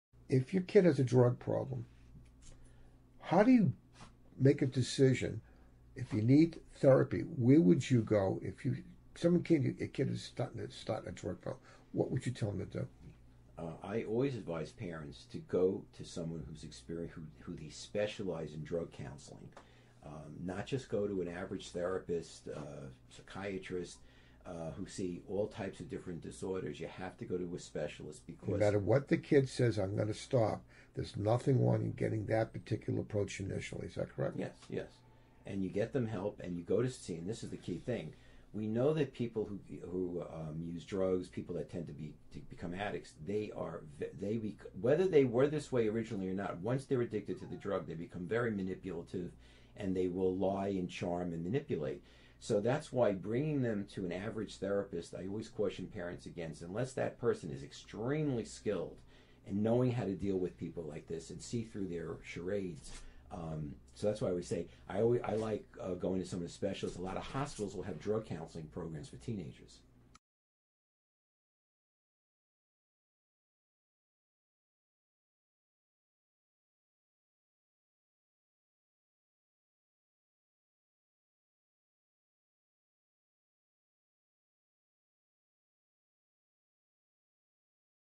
Drug Addiction - What Parents Should Do First
Discussion with Steven Sussman, Ph.D -  Child and Adolescent  Psychologist  Co-Founder of the Child and Teen Success Centers in  NY and NJ